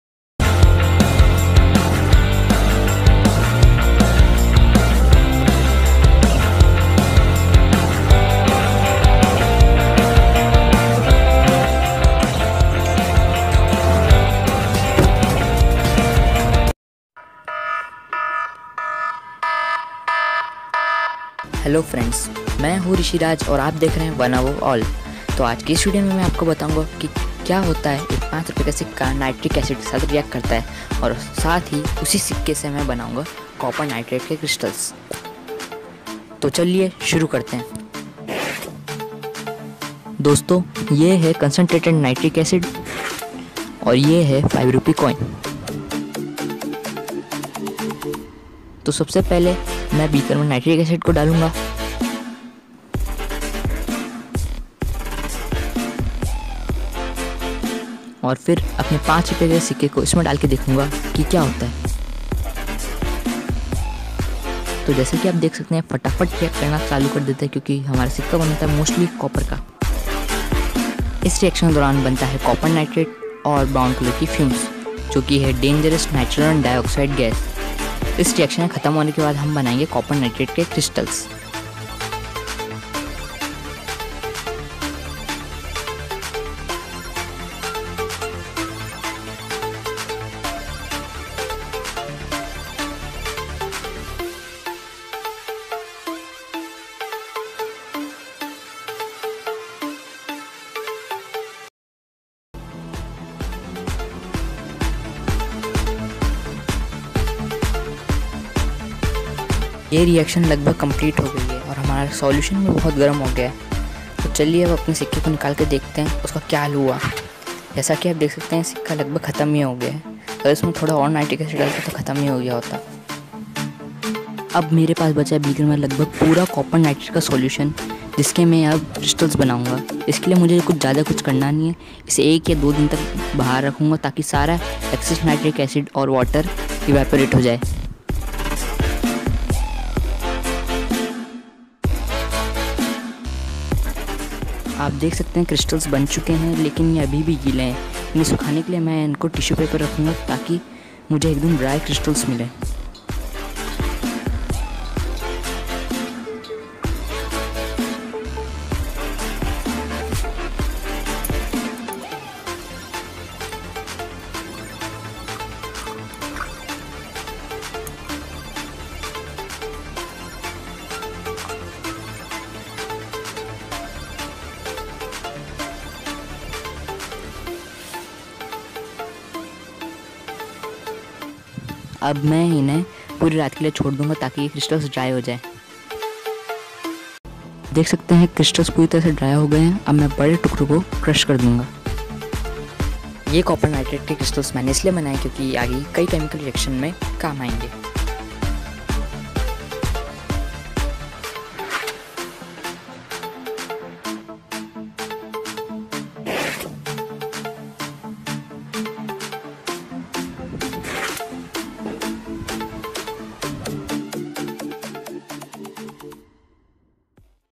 Nitric acid (तेजाब) vs 5 rupee coin and then making its crystals.
In this video I will do a experiments
Nitric acid vs 5 rupee coin.